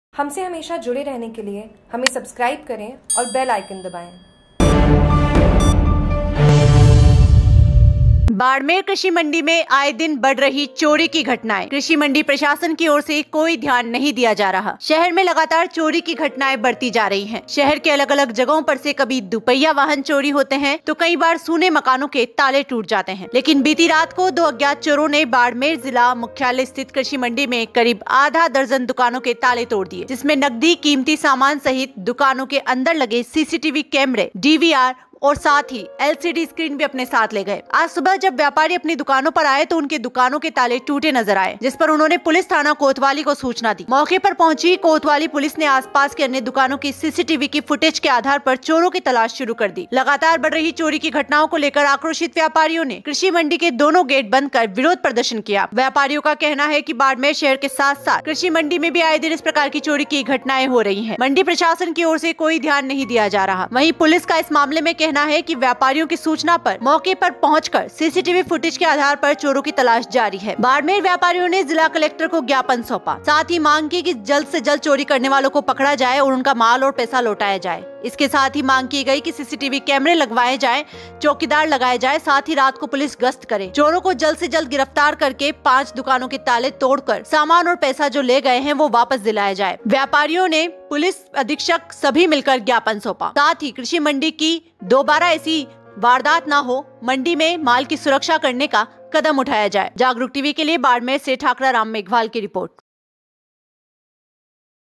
कृषि मंडी में आए दिन बढ़ रही चोरी की घटनाएं, कृषि मंडी प्रशासन की ओर से कोई ध्यान नहीं दिया जा रहा।
शहर में लगातार चोरी की घटनाएं बढ़ती जा रही है. शहर के अलग-अलग जगहों पर से कभी दुपहिया वाहन चोरी होते हैं तो कई कई बार सुने मकानों के ताले टूट जाते हैं लेकिन बीती रात को दो अज्ञात चोरों ने बाड़मेर जिला मुख्यालय स्थित कृषि मंडी में करीब आधा दर्जन दुकानों के ताले तोड़ दिए. जिसमें नकदी कीमती सामान सहित दुकानों के अंदर लगे सीसीटीवी कैमरे डीवीआर एलसीडी स्क्रीन भी अपने साथ ले गए. आज सुबह जब व्यापारी अपनी दुकानों पर आए तो उनके दुकानों के ताले टूटे नजर आए जिस पर उन्होंने पुलिस थाना कोतवाली को सूचना दी. मौके पर पहुंची कोतवाली पुलिस ने आसपास की अन्य दुकानों की सीसीटीवी की फुटेज के आधार पर चोरों की तलाश शुरू कर दी है. लगातार बढ़ रही चोरी की घटनाओं को लेकर आक्रोशित व्यापारियों ने कृषि मंडी के दोनों गेट बंद कर विरोध प्रदर्शन किया. व्यापारियों का कहना है कि बाड़मेर शहर के साथ-साथ कृषि मंडी में भी आए दिन इस प्रकार की चोरी की घटनाएं हो रही है. कृषि मंडी प्रशासन की ओर से कोई ध्यान नहीं दिया जा रहा है. वहीं पुलिस का कहना है कि व्यापारियों की सूचना पर मौके पर पहुंच सीसीटीवी फुटेज के आधार पर चोरों की तलाश की जा रही है।"बाडमेर व्यापारीयो ने जिला कलेक्टर को ज्ञापन सोपा माँग जव्द जल्द चोररी करने वालो  को पकडे हमारा चोरी किया माल व पैसा हमे लोटाये कुर्षी मण्डी सी सी टी कैमरे लगावे चौकीदार लगावे पुलिस रात गस्त करे  चोरों को जल्द से जल्द गिरफ्तार करके पांच दुकानों के ताले तोड़कर सामान और पैसा ले गए हैं हमें दिलाएं व्यापारियों ने पुलिस अधीक्षक सभी मिलकर ज्ञापन सौंपा कृषि मंडी की - दुबारा ना हो हमारे मंडी में माल की सुरक्षा करने का कदम उठाया जाएं